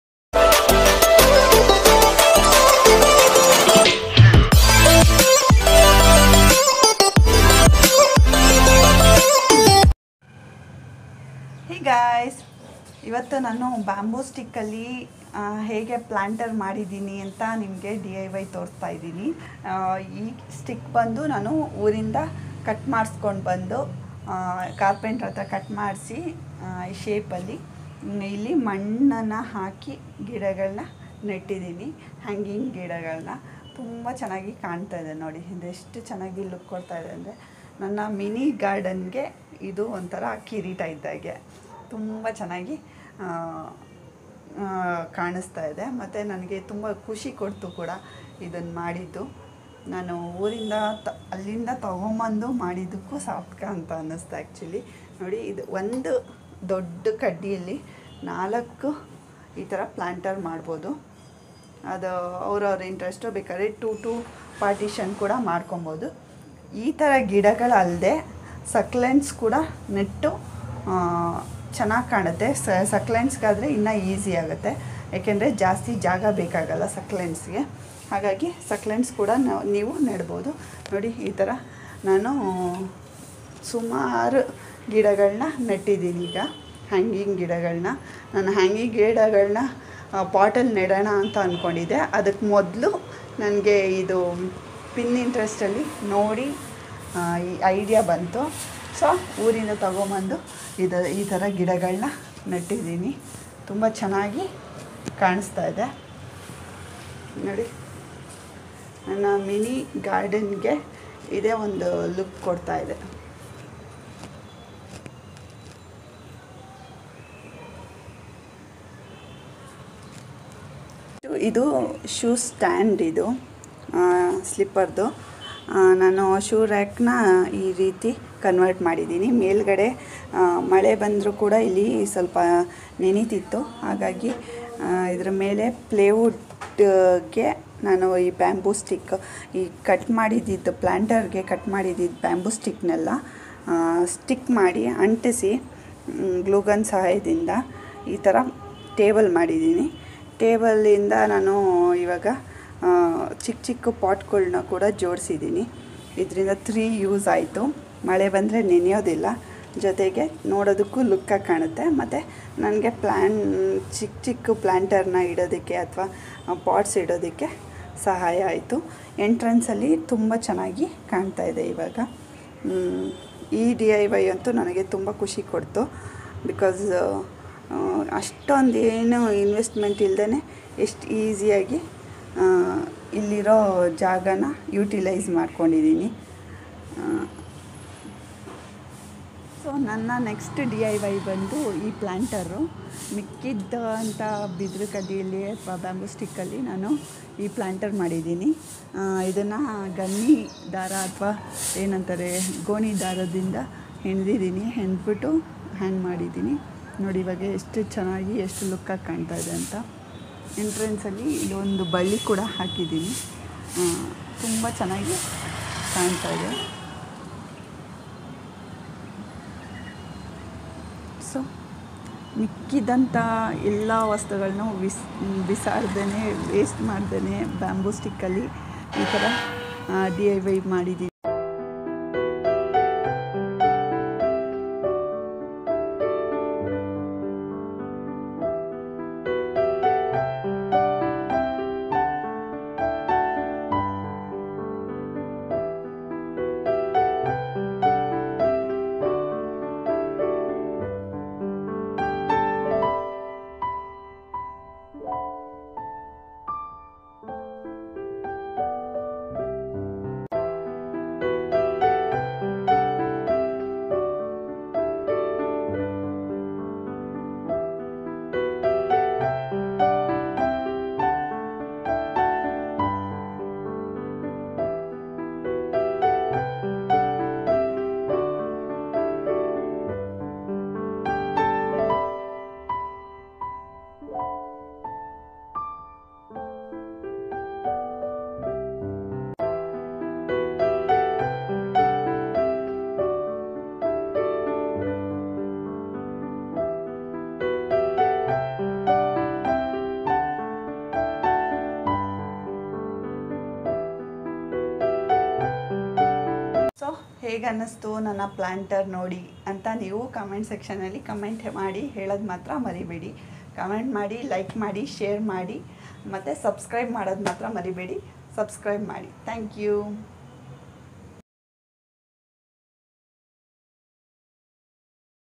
indoor plant |DIY planter with bamboo |gardening | beautiful home decor |kannada vlog| home minister
planter made using bamboo sticks which gives a wonderful outstanding look to our garden